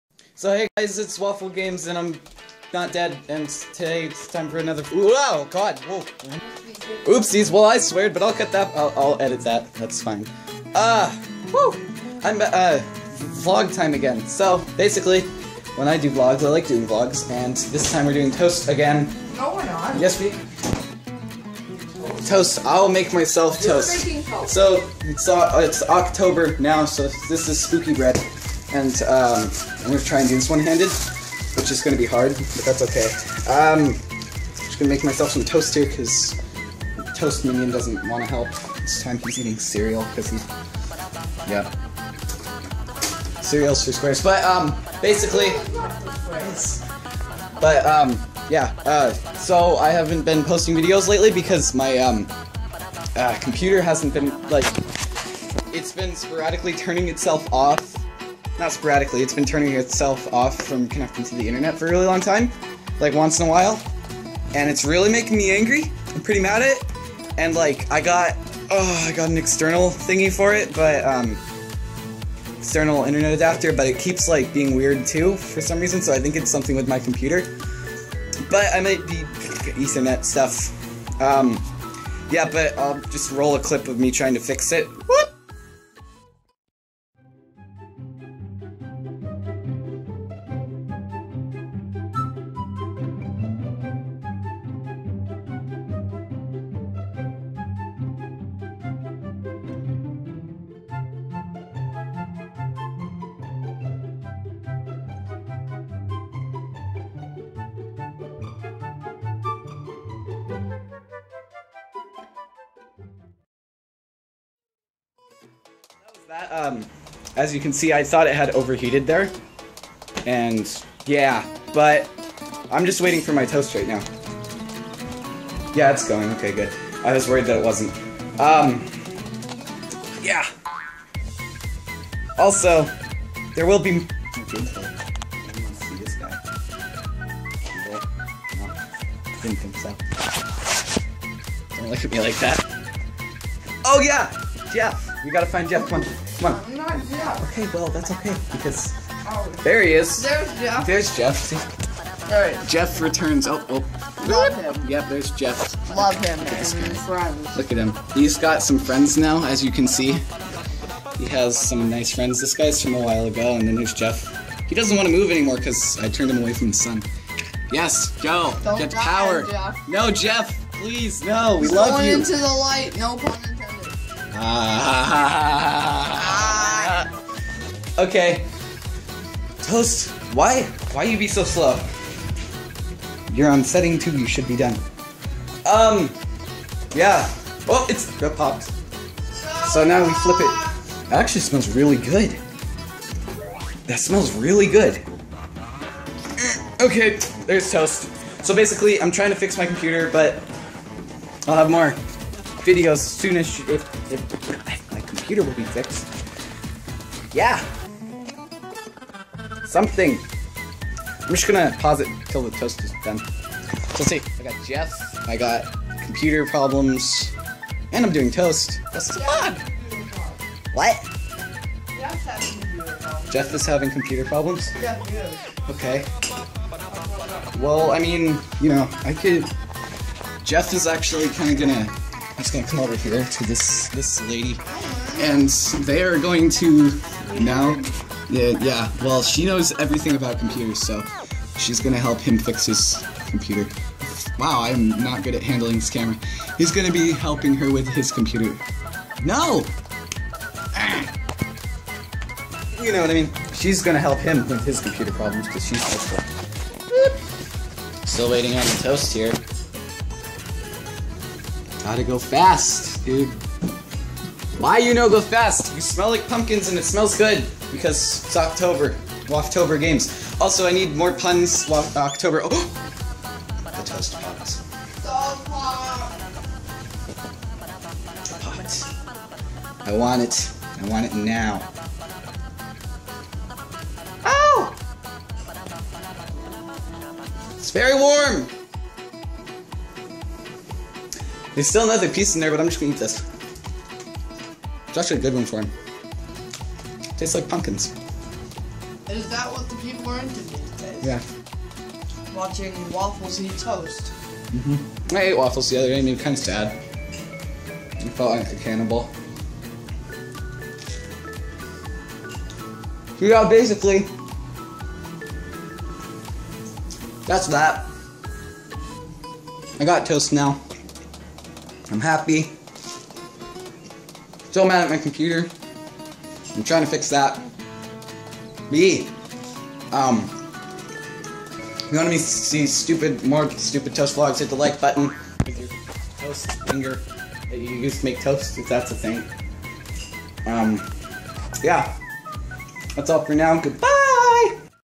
Toast vLog 2: I'm alive!
So yeah my computer is super evil right now, but hey, i might have a fix. More videos soon. blah blah, love you guys.